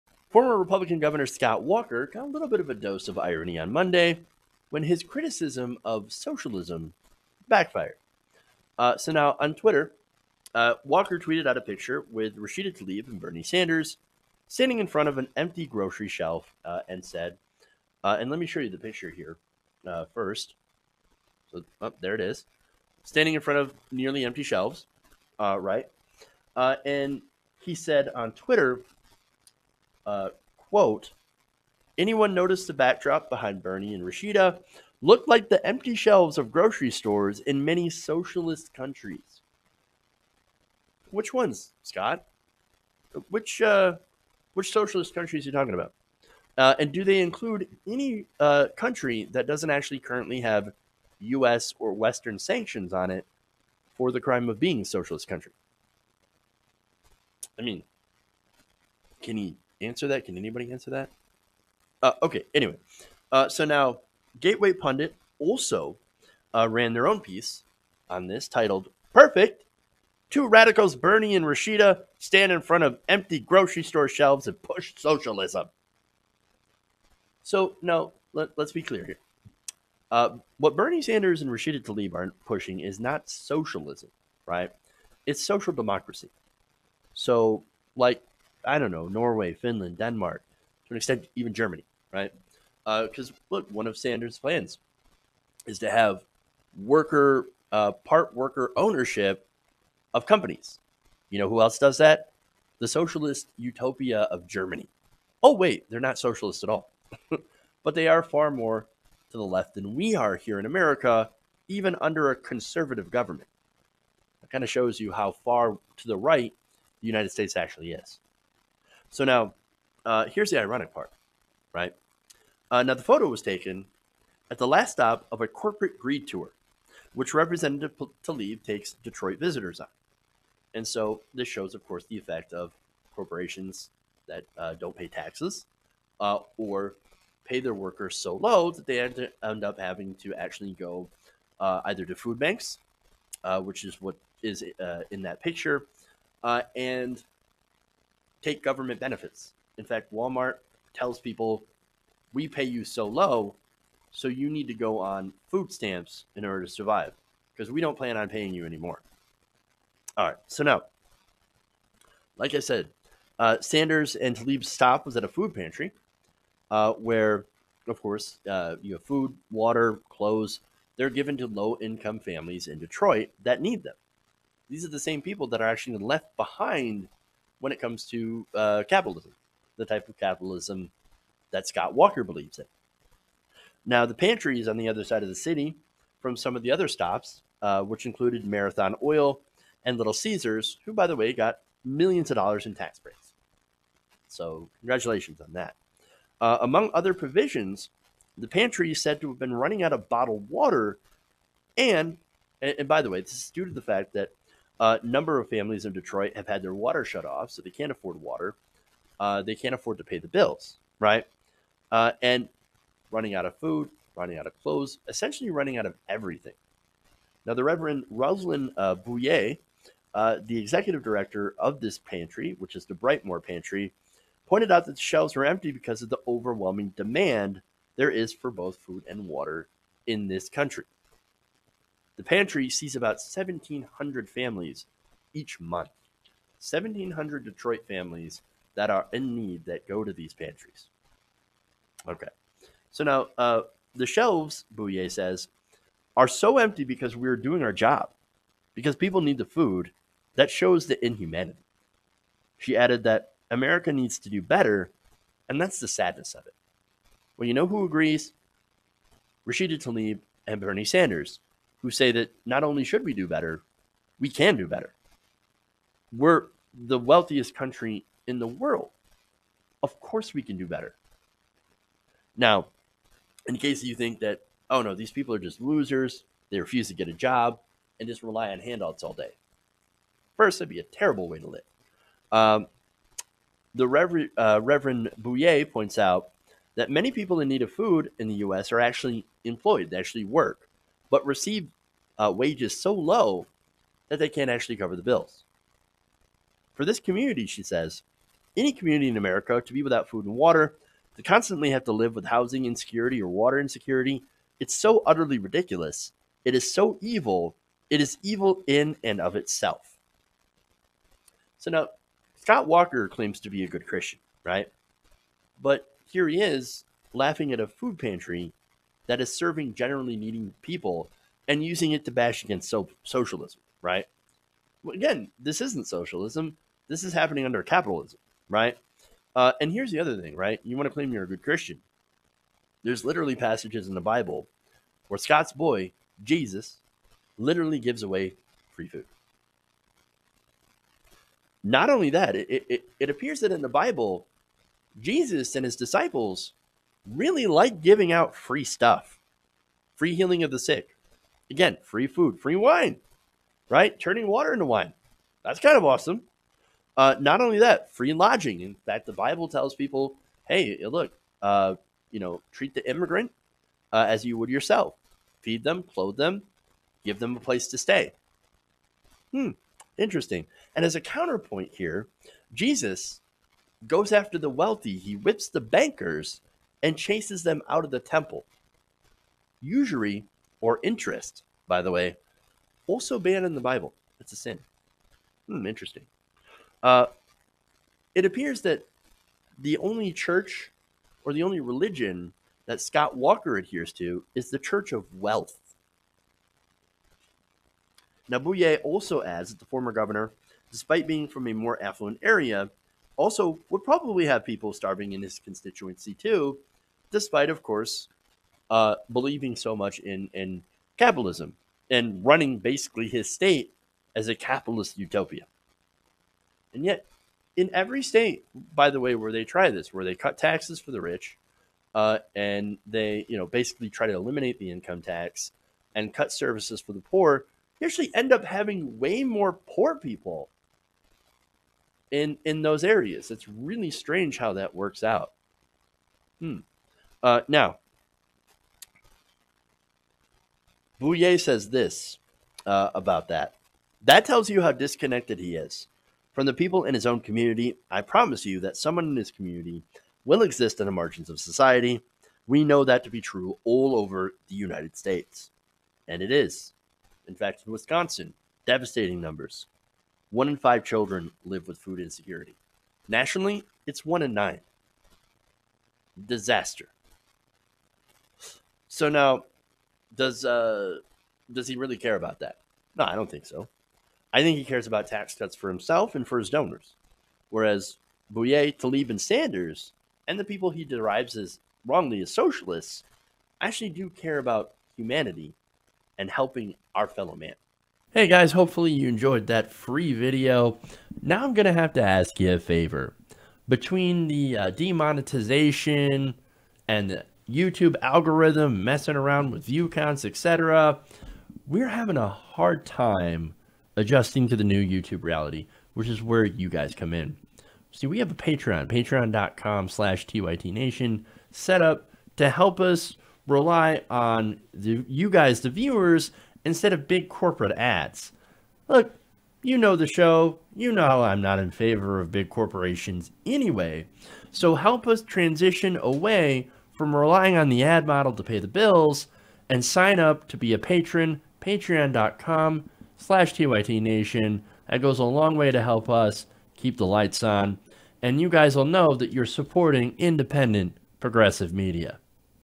Scott Walker's Socialism Tweet Hilariously Backfires
Follow Jeff on Twitter: @JeffsPolitics